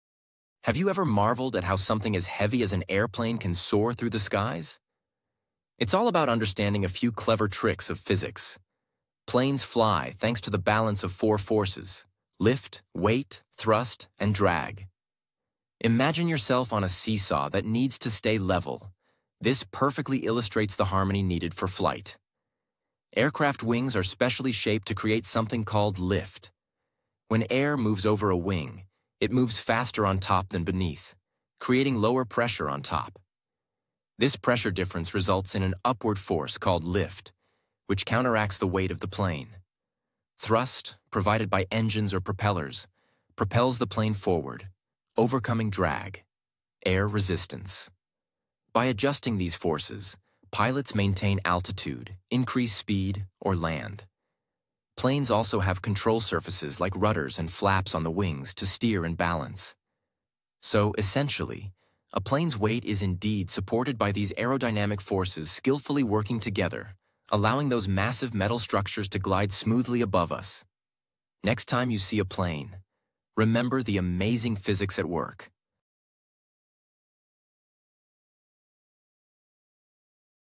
ELI5: Why Do Planes Fly Despite Being Heavy?
Discover the secret behind how gigantic metal birds soar effortlessly through the skies! ✈️